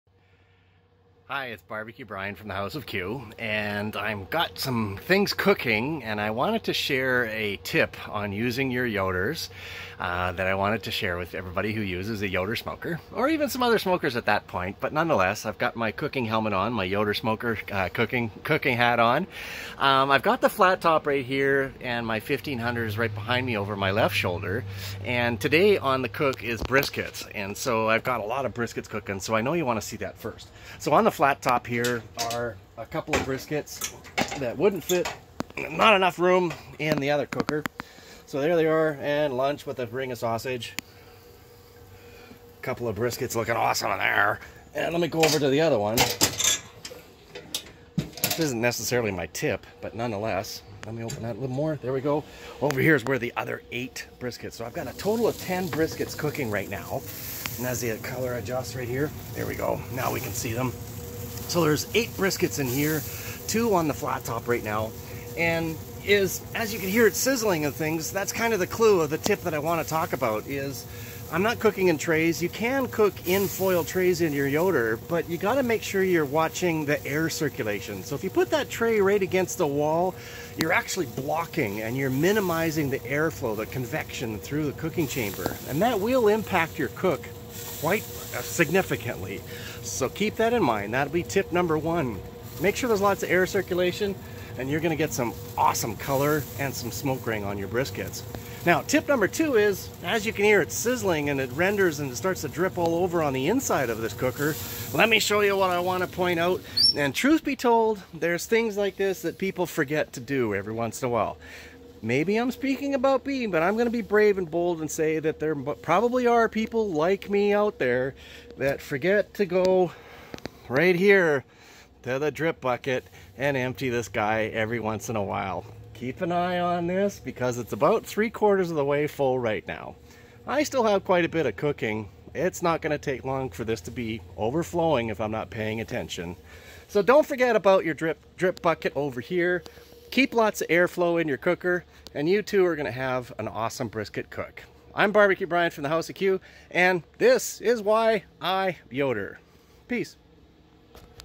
Two tips to cooking on a Yoder Smoker
Brisket's are being cooked today but two tips from BBQ Brian of House of Q to master your smoker.